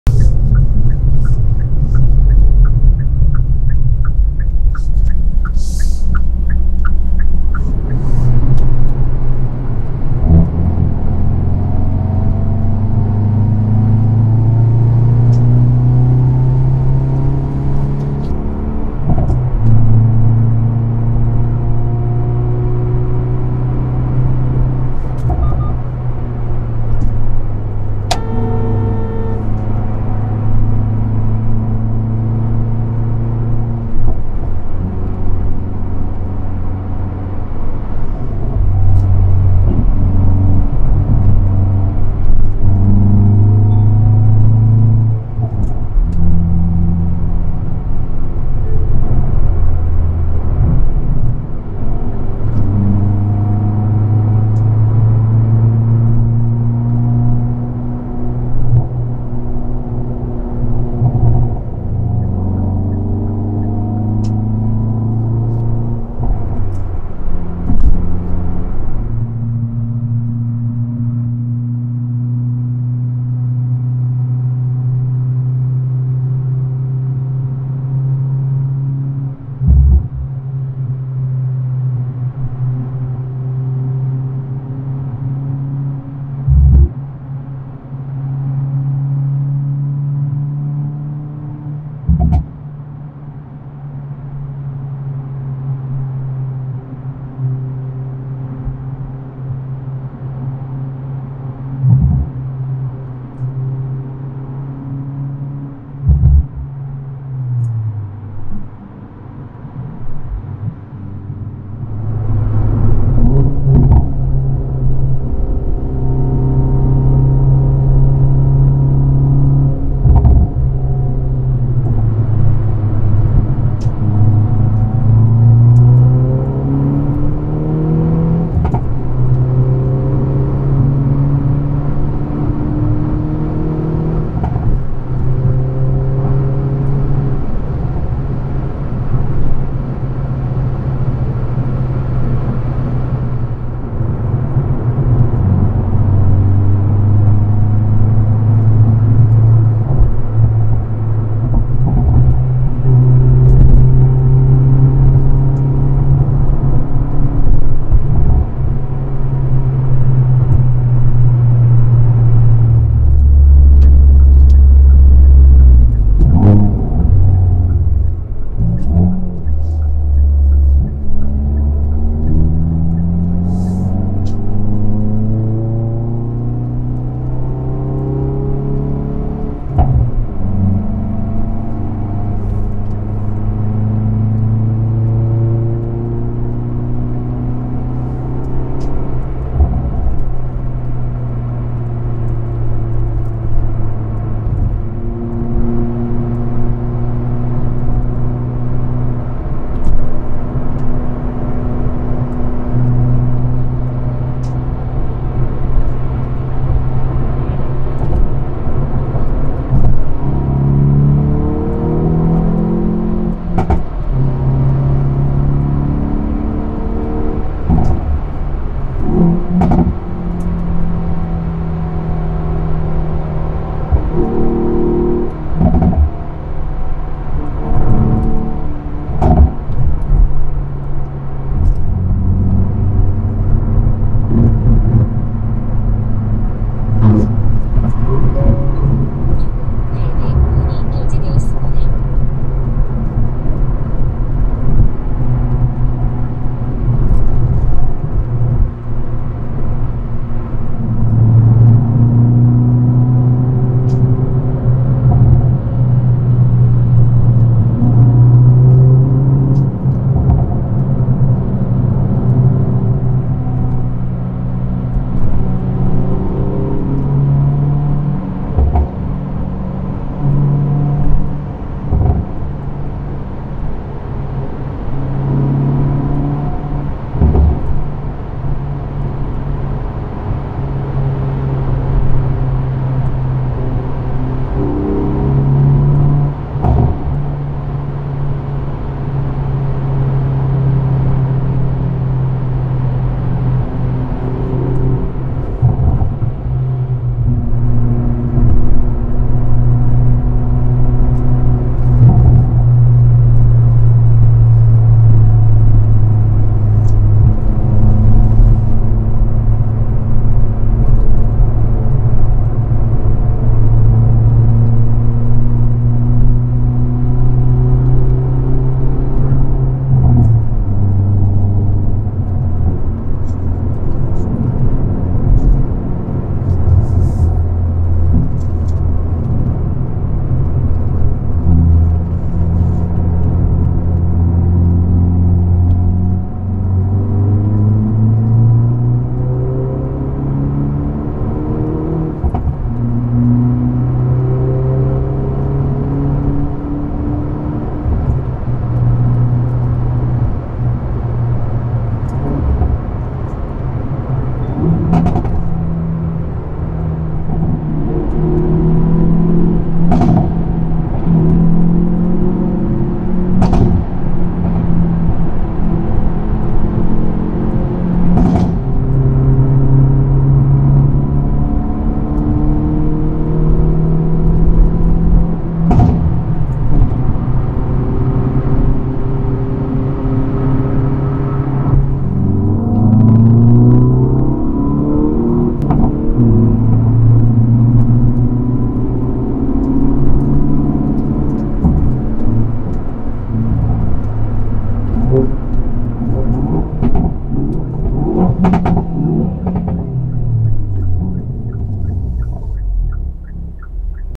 [Driving] 포근해서 나온 드라이빙 (Veloster N) Daily driving_Bundang to Dasan (Round trip)
[20200222]

오래간만의 드라이빙
분당-다산 왕복 일상주행
현대 벨로스터N (퍼포먼스패키지 / 275HP)
주행모드: 커스텀
                  (엔진,배기,ESC: 스포츠플러스, 서스: 노멀 스티어링휠: 스포츠)

Night driving
Daily Driving in a mild winter
Hyundai Veloster N (Performance package / 275HP)
Driving mode: Custom

「Camera」
Model: Sony α6400, DJI osmo action
Resolve: 4K/UHD(3840x2160) / 24fps
Audio: rode videomicro
Color grading: X